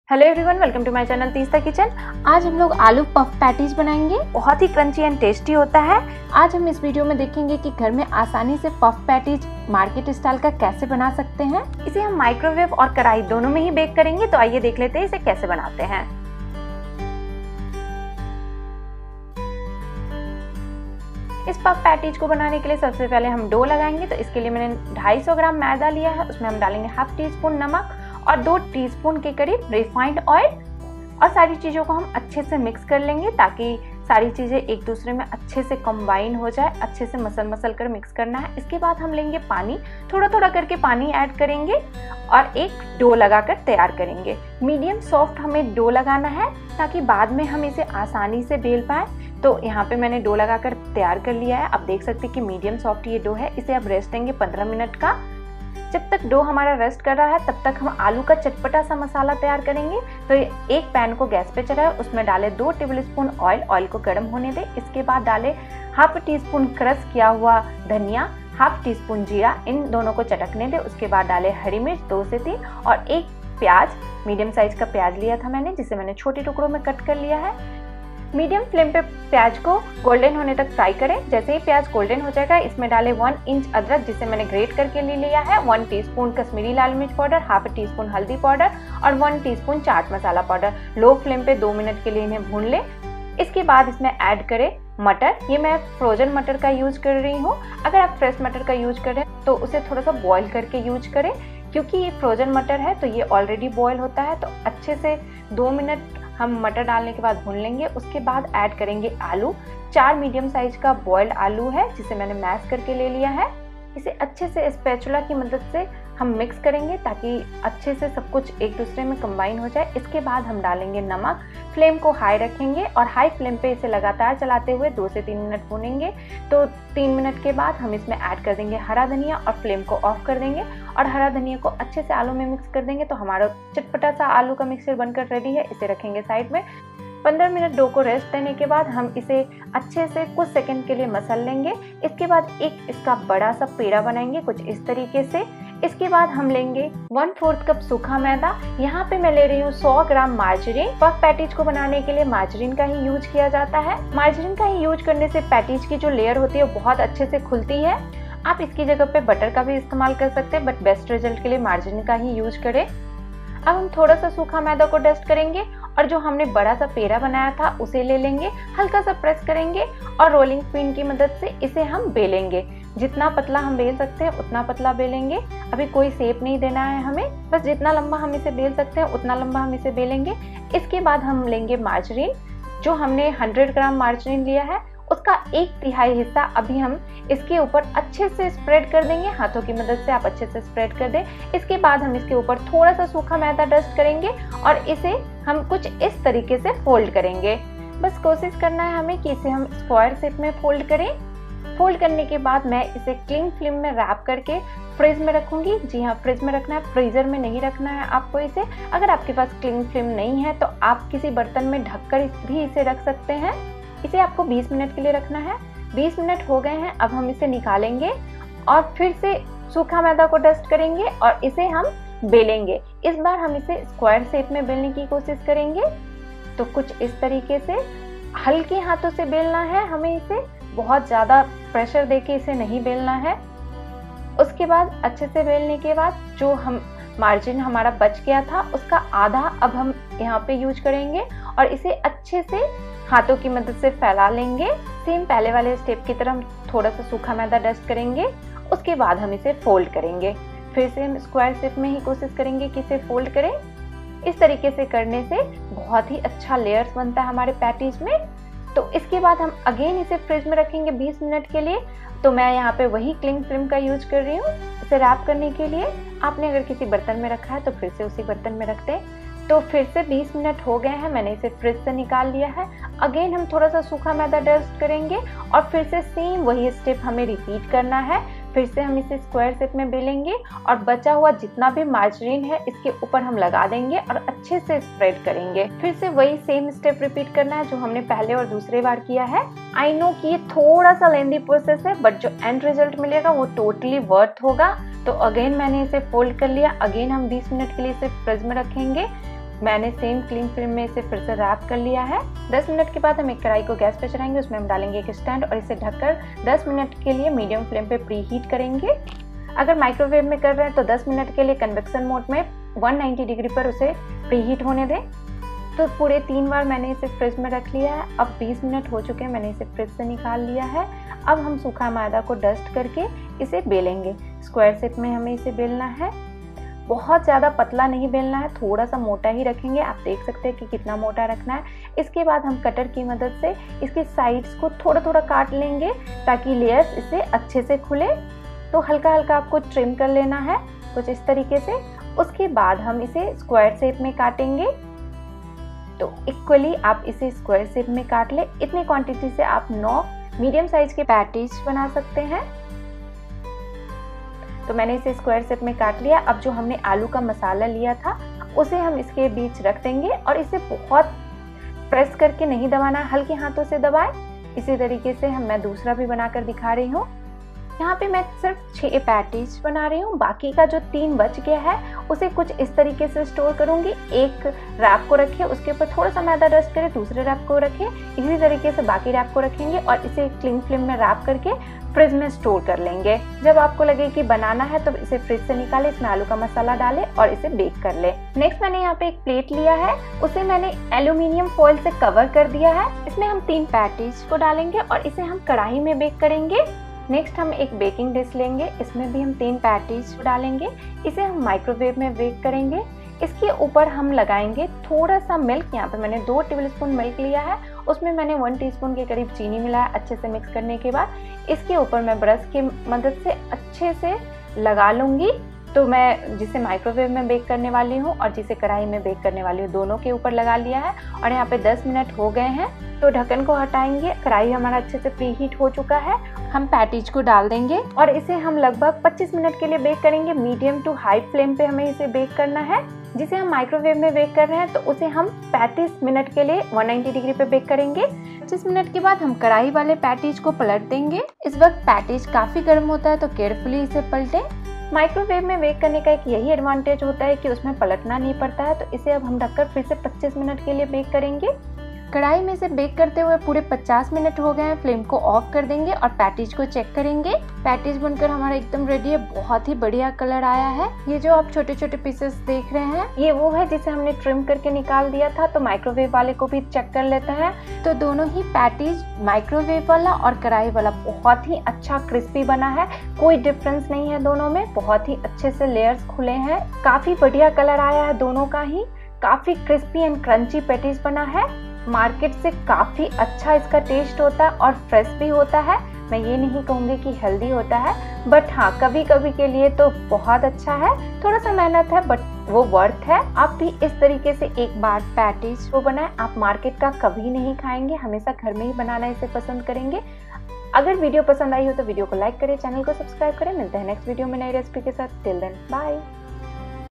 Puff Patties Recipe without oven | Bakery Style Puff Pastry | अनगिनत परतो वाली क्रिस्पी पफ पैटीज
In this video you will see puff patties recipe | very simple and easy method of making this aloo patties